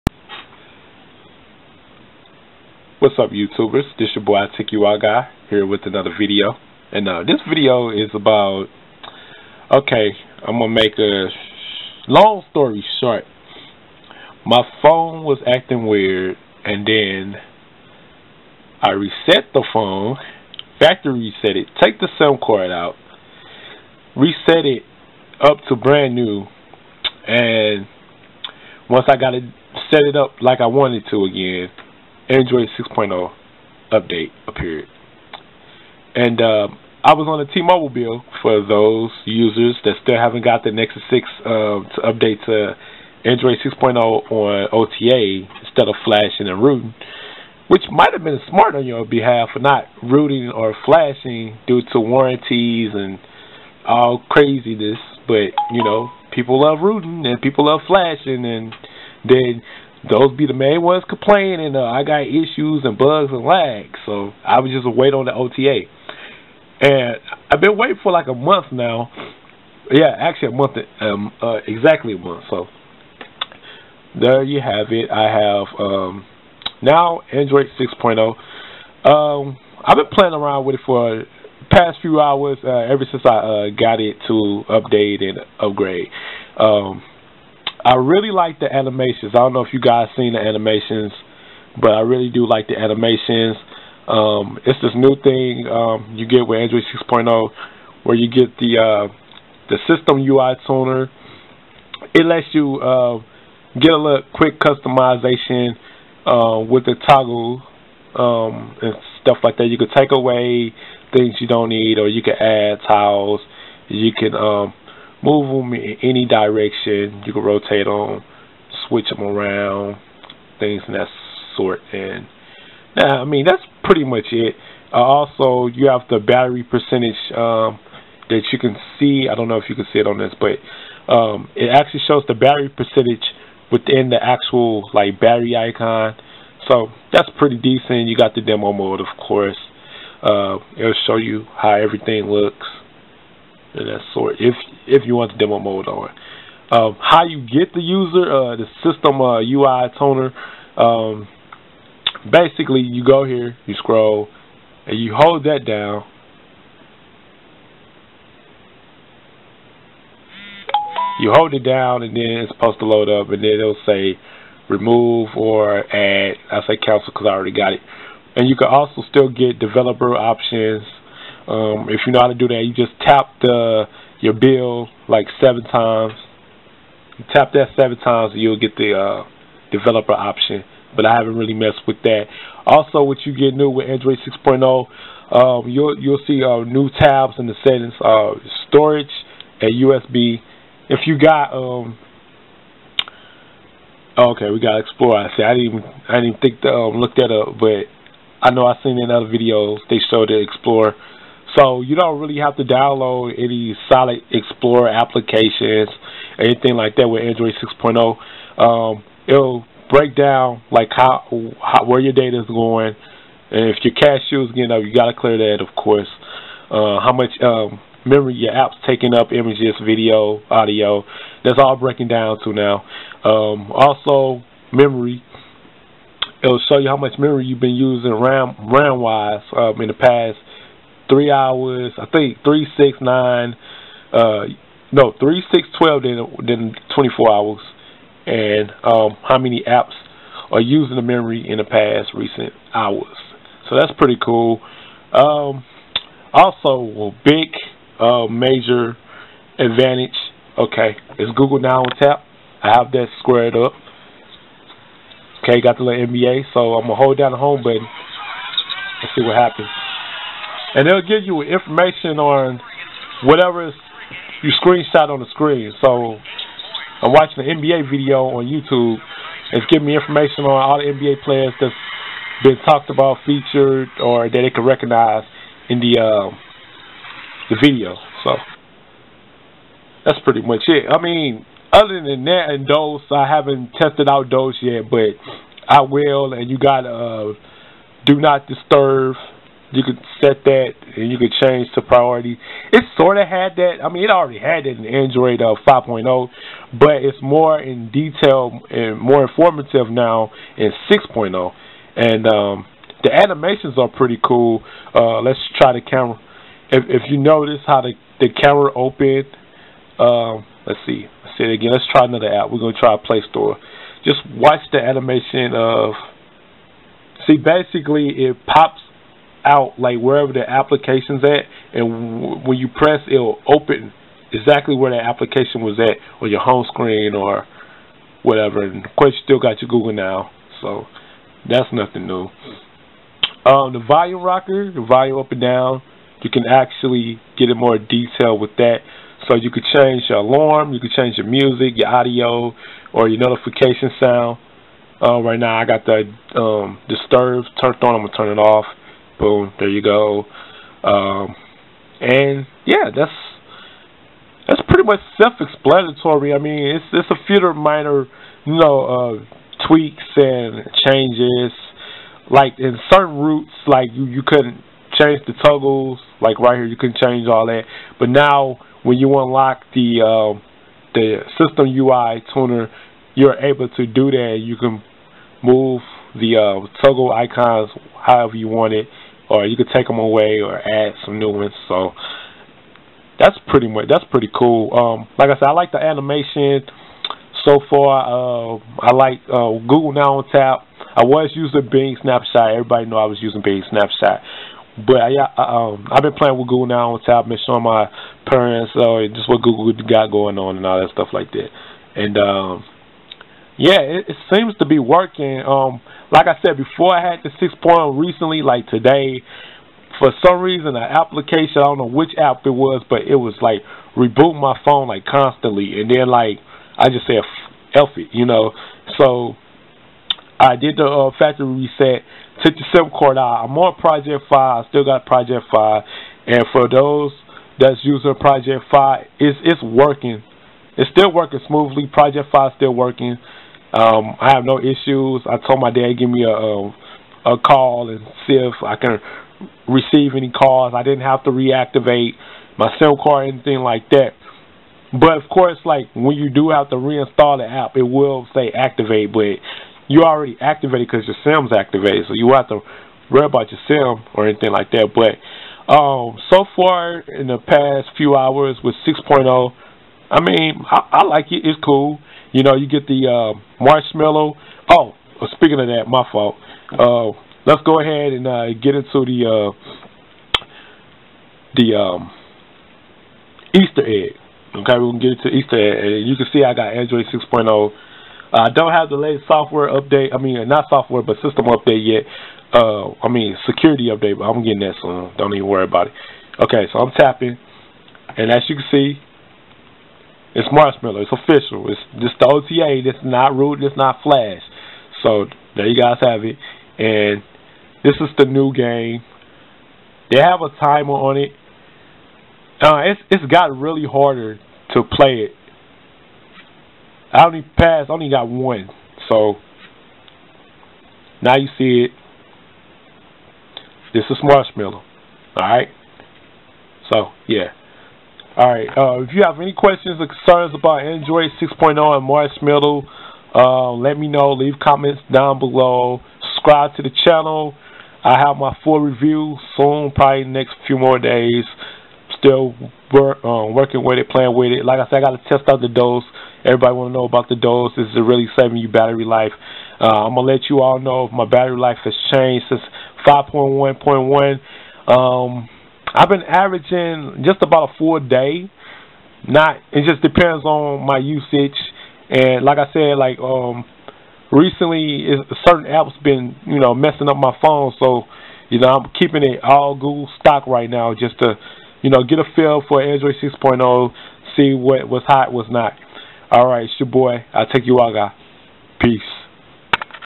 Nexus 6 Android 6.0 Marshmallow OTA Version (Overview)
hey everyone. This is my mini review of the Android 6.0. I know most users have gotten this already through root and flash.  But for the ones that waiting on the OTA, I have gotten it by factory resetting my Nexus 6 and ejecting my sim card too.  I was also on the T-Mobile bill as well, for those that wanted to know which bill I was on.  As soon as signed back in to my gmail account and reached the homescreen setup, I saw the OTA for Android 6.0 appeared.  So I quickly upgraded. lol. But I also using Project Fi and once I setup the app again and inserted my old sim, everything worked normally. For those of you who are worried about Project Fi sim card not working after setting up Android 6.0 OTA. Maybe with Flashing and rooting it was giving people issues. But I have no problems.  So far from the past few hours of using Android 6.0 Marshmallow, it has been cool.  Its nothing to be shouting for joy like you hit the lotto, but its been decent.  What I like about it is the System UI Tuner option available out the gate.  You can change how you arrange the icons in the toggle and you also can see the percentage of the battery in status. Some small things that root access or other Android OS UIs would let you do like in CM and OnePlus. Also Google Now On Tap is available in 6.0. This will allow you to gather more information about a particular subject or topic or person and place. Also another thing i like about 6.0 is the little small animation changes with apps opening and closing.  I also liked the fact that Memory management is more organized and you can see exactly where your memory comes from.  So you can see in detail the apps are using up memory and where the RAM is going.  Not only that, you also will see Google Settings is inside the main phone settings section as well.  Apps Permission I forgot to mention in this video is also here in Android 6.0.  This allows you to take full control of permissions of each app. You can pick whether the app should be allowed to use microphone, camera, etc.  The volume rocker now shows audio, alarm and notification sound levels.  So you have full control of your volume levels as well. Pretty decent.  Another minor adjustment is the do not disturb mode. Now, this was always in Android 5.0 but it is a little more detail with information and it is more visible to use.  So you can choose how you want the Do Not Disturb settings to work for you.  Last thing I want to talk about is Doze.  This can be setup in the phone settings under the Battery tab.  Your apps are optimized for this at first, so you will have to go into battery settings to configure Doze.  Now I wasn't able to demo this off in video, but it works best when your phone or device isn't in use. You will notice the difference of battery usage once your phone has been sitting idle for a while. Not so sure if this has improved battery life overtime.  But will do a different video blog for Doze.  Well, this will be all for me to share with you all about my experience with Android 6.0 Marshmallow so far.  Will do a full in-depth video soon.